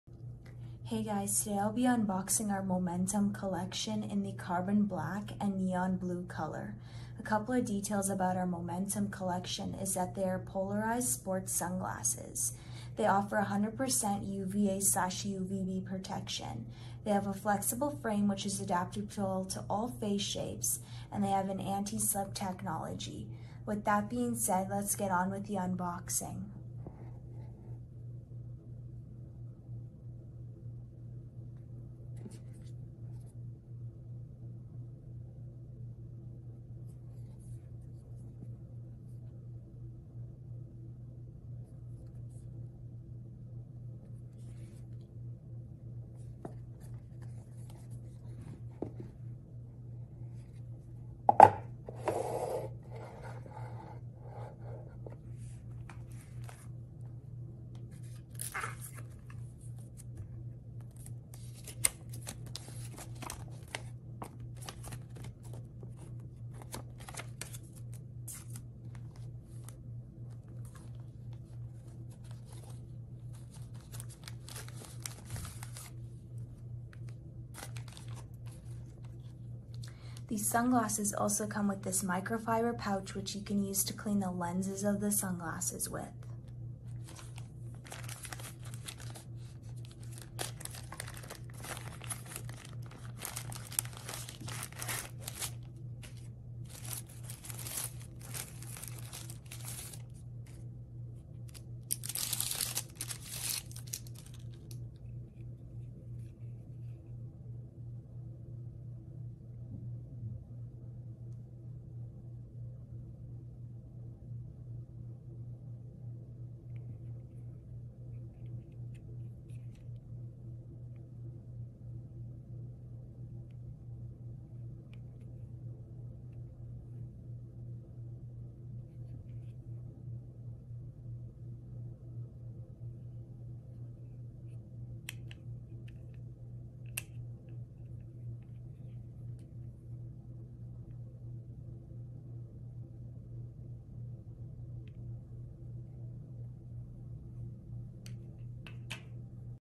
Momentum Polarized Sports Sunglasses in the Carbon Black and Neon Blue Colour- Unboxing
Here is the unboxing of our Momentum polarized sunglasses in the carbon black and neon blue colour.